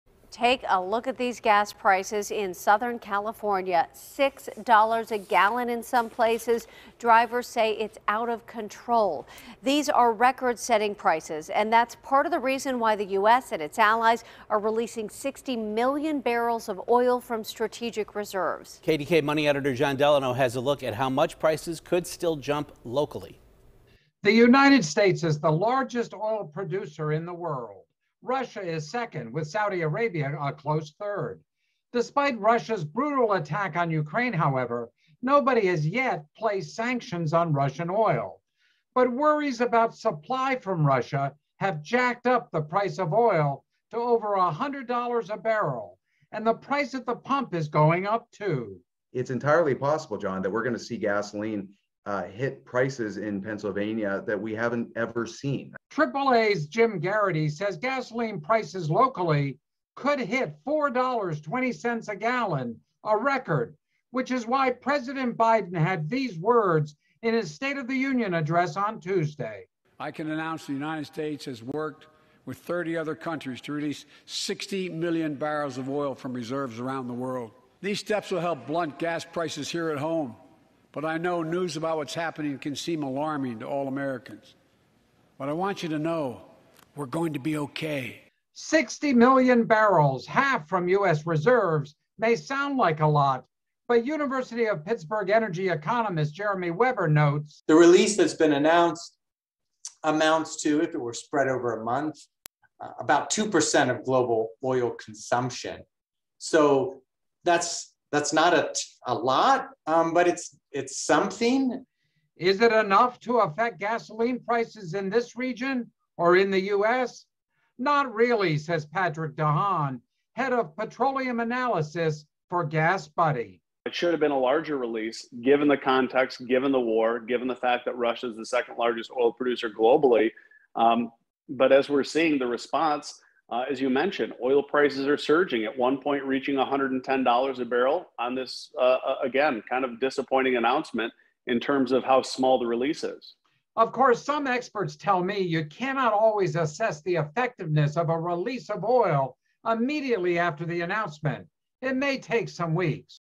Release Of Oil Reserves May Do Little To Blunt Gasoline Price Hikes
Despite Russia’s brutal attack on Ukraine, however, nobody has yet placed sanctions on Russian oil.